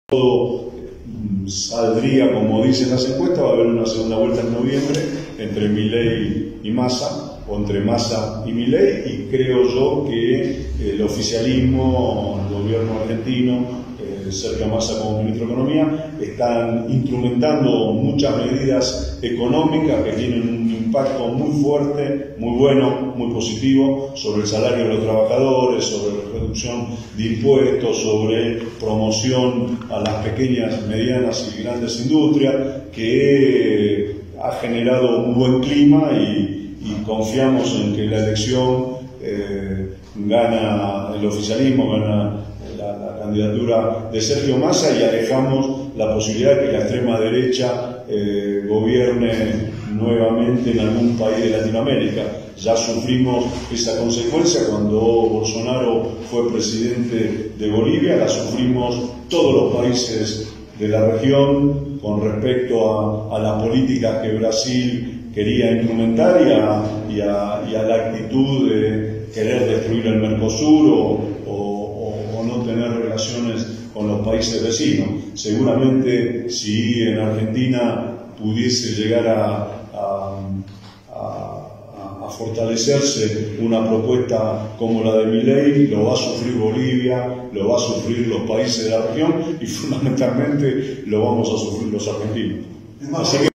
Embajador Basteiro analiza las elecciones en Argentina y sus implicaciones para la región
📌 El embajador de Argentina 🇦🇷 en Bolivia 🇧🇴, Ariel Basteiro, informó que las encuestas apuntan que las elecciones en el vecino país llegarán a una segunda vuelta entre el candidato oficialista Sergio Massa y el derechista radical Javier Milei.

En ese punto, dijo que será decisivo el apoyo a las pequeñas, medianas y grandes empresas mediante la reducción impositiva que generaron un buen clima que propuso Massa en el debate con miras a las elecciones. Además, recordó los obstáculos para la región generados por exmandatarios de la extrema derecha, como Jair Bolsonaro, en Brasil 🇧🇷.